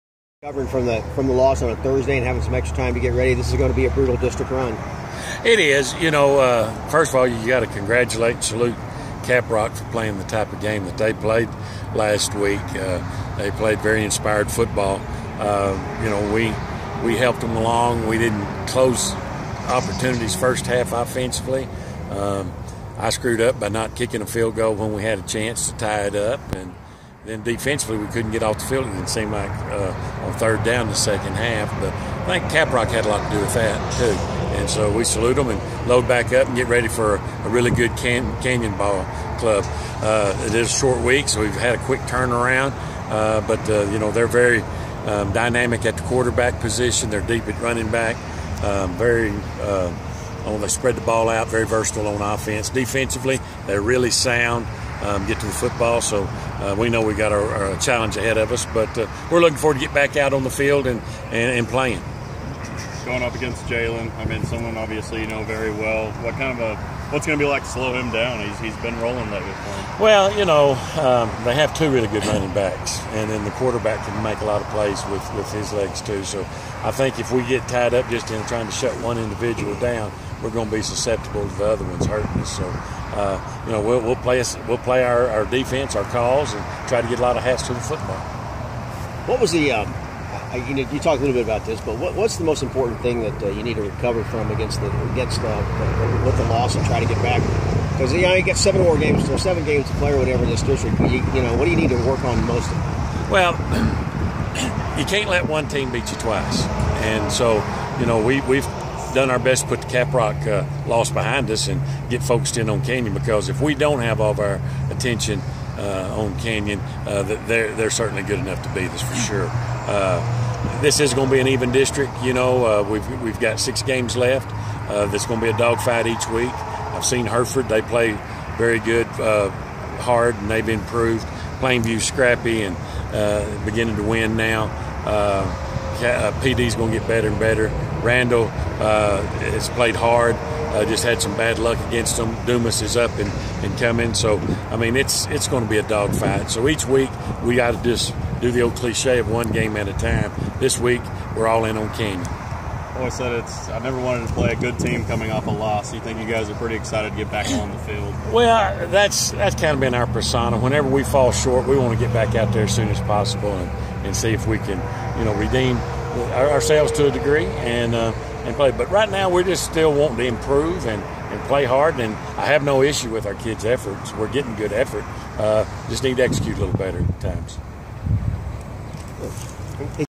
Sandies coach Mel Maxfield
Didn't play well enough to beat Caprock